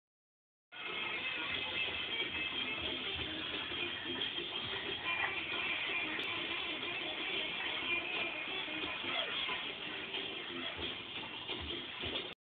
Ryan dancing to Pee Wee Herman.
Ryan, 3 years old, dancing to Pee Wee Herman.